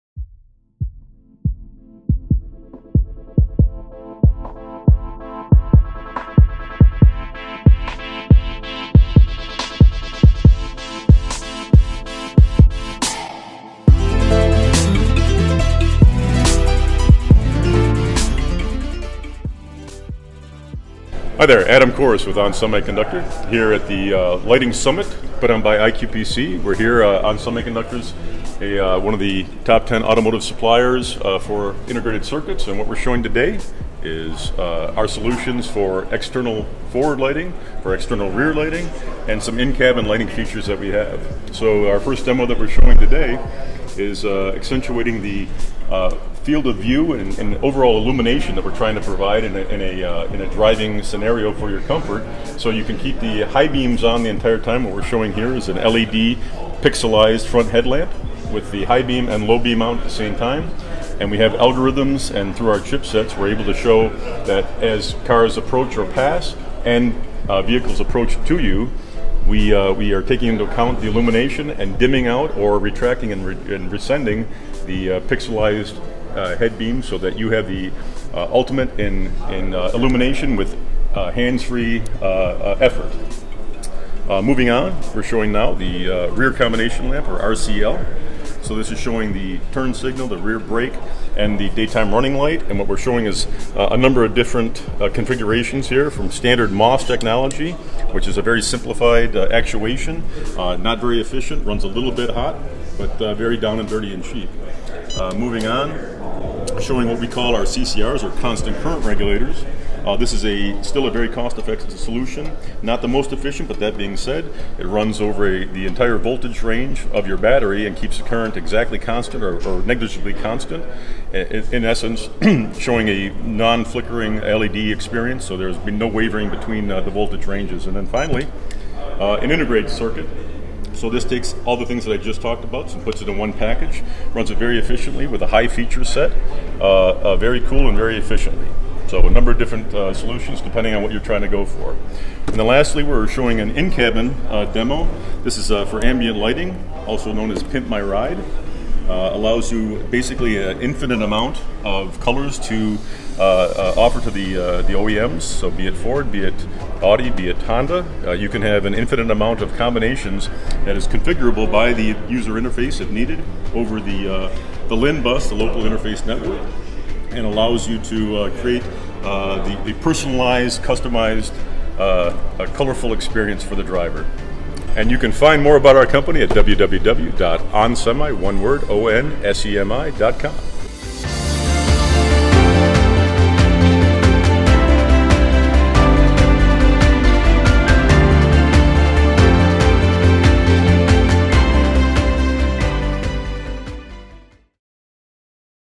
ON Semiconductor Interview at IQPC Advanced Lighting for Automotive Summit 2017
IQPC holds a conference every year geared towards lighting designers, engineers, suppliers and manufacturers to share ideas and discuss new lighting trends in the automotive industry. This conference is called Advanced Lighting for Automotive Summit.